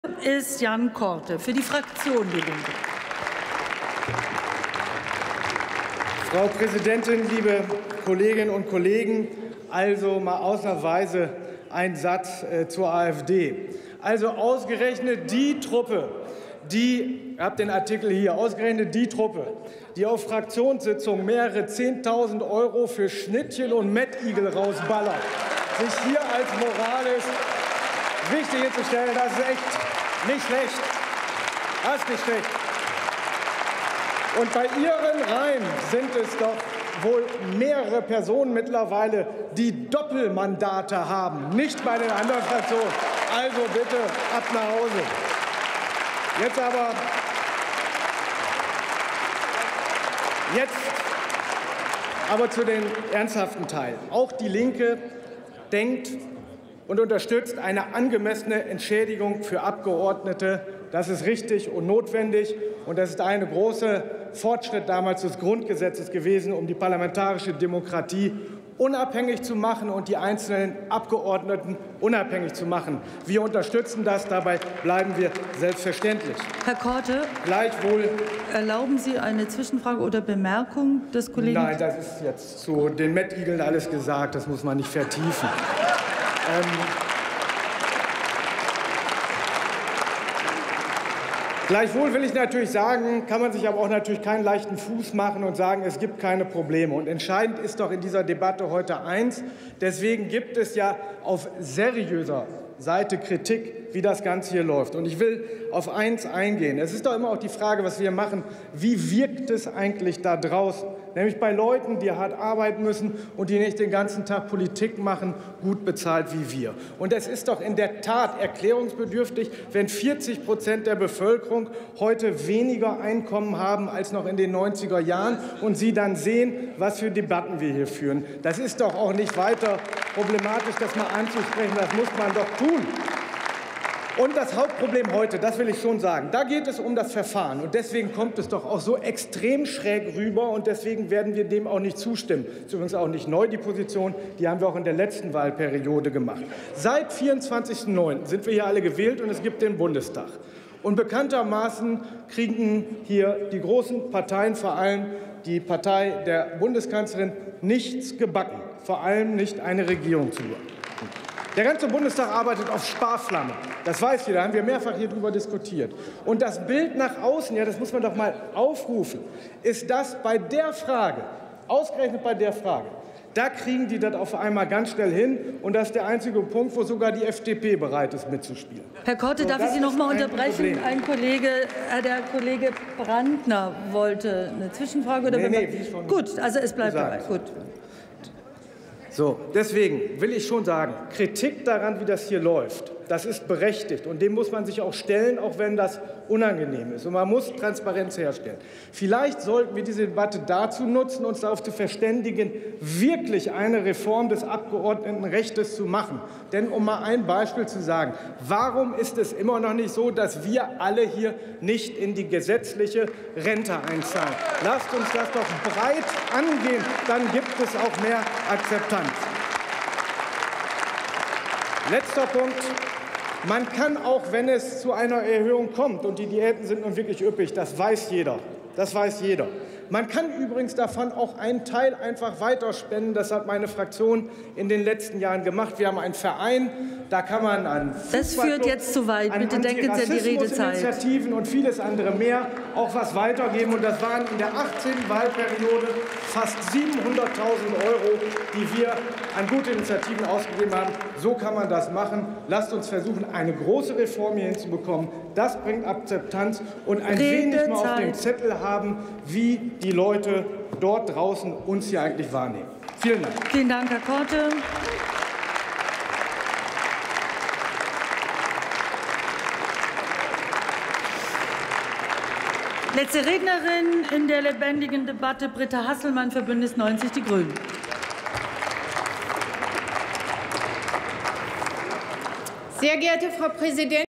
Jan Korte, DIE LINKE: Statt Diätenerhöhung eine wirkliche Reform des Abgeordnetenrechts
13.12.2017 - Ausnahmsweise mal ein Wort zur AfD: Ausgerechnet die Truppe, die auf Fraktionssitzungen mehrere zehntausend Euro für Schnittchen und Mettigel rausballert, stellt sich in der Diäten-Debatte als moralisch wichtig hin. Und in den Reihen der AfD haben mehrere Personen Doppelmandate - nicht in den anderen Fraktionen. DIE LINKE ist für eine angemessene Entschädigung für Abgeordnete, um die parlamentarische Demokratie und die einzelnen Abgeordneten unabhängig zu machen. Es ist aber in der Tat erklärungsbedürftig, wenn 40 Prozent der Bevölkerung heute weniger Einkommen haben als noch in den 90er Jahren. DIE LINKE ist für mehr Transparenz und eine wirkliche Reform des Abgeordnetenrechts. Warum ist es immer noch so, dass wir alle hier nicht in die gesetzliche Rente einzahlen? DIE LINKE hat in der letzten Wahlperiode 700.000 Euro aus der Diätenerhöhung gespendet und wird das fortsetzen.